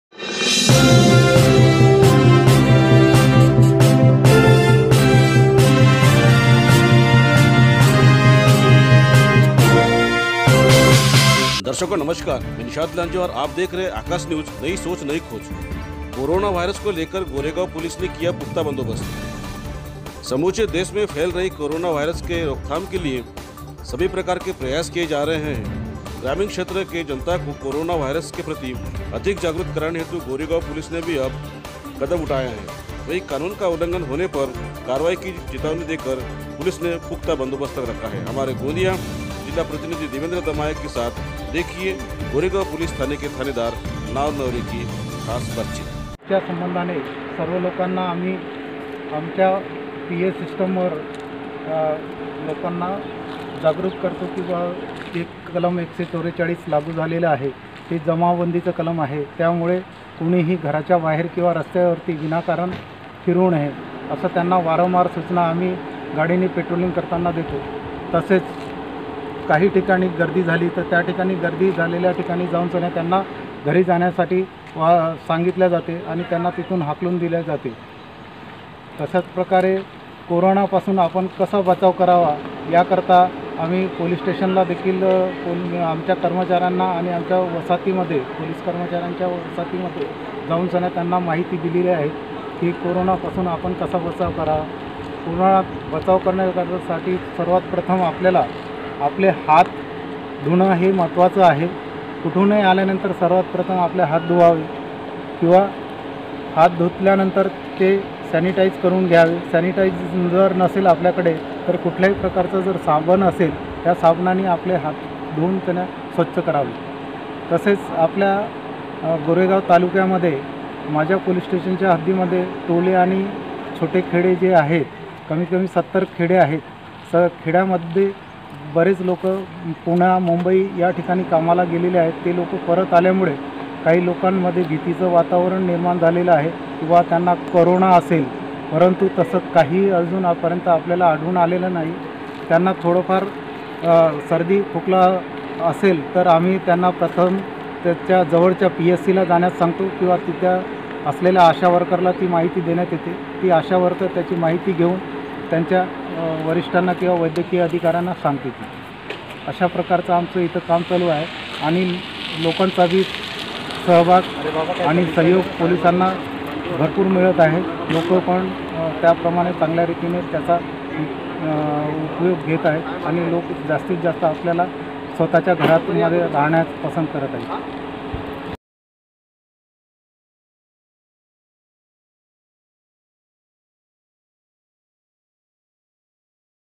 COVID-19 के जनजागृति के लिए गोरेगांव पुलिस का पुख्ता बंदोबस्त।
@ AKASH NEWS #

मुख्य संपादक निशाद लांजेवार ( ९३५९२४४१०५ )